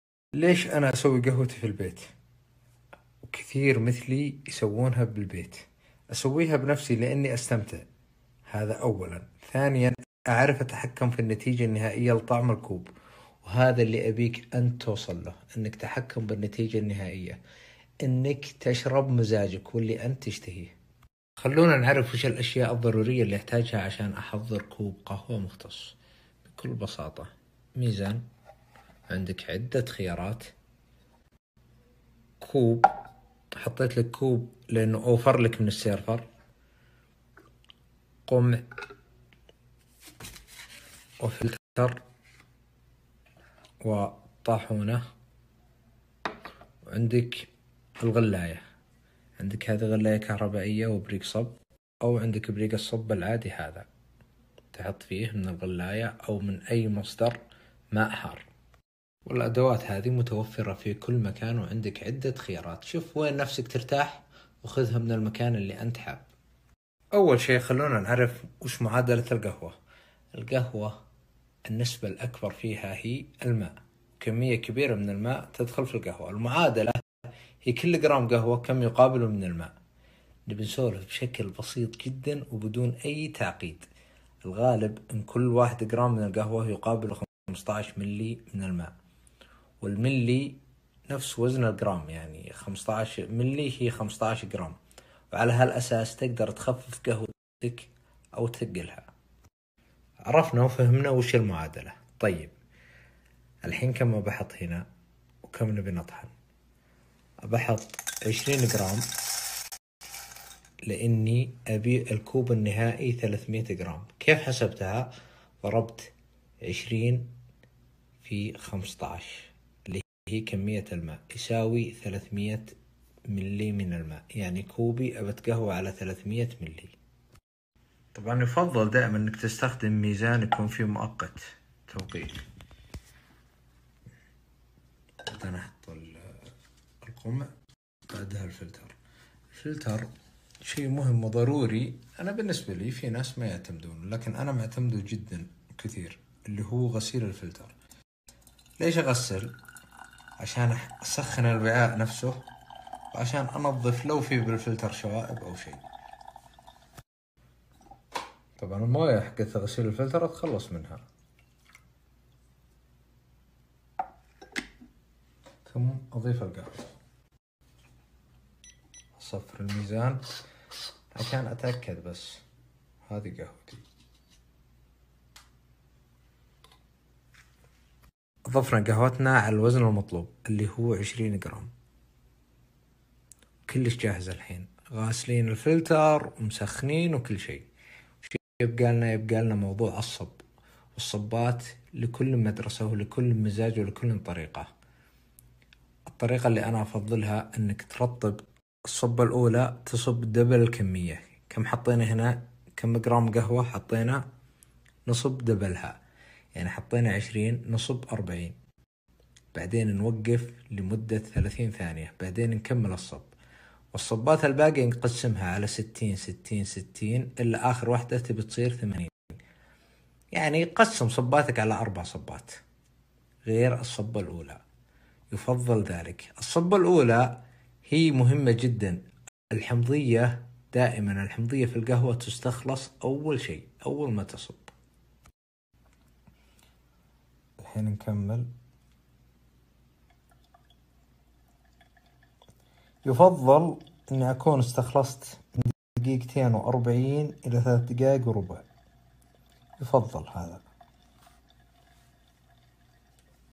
تحضير القهوة المقطرة من المنزل بشكل مبسط من سناب ثامر
تحضير القهوة المقطرة
قهوة ترشيح
قهوة فلتر
القهوة المقطرة للمبتدئين

سناب ثامر
thamerjr1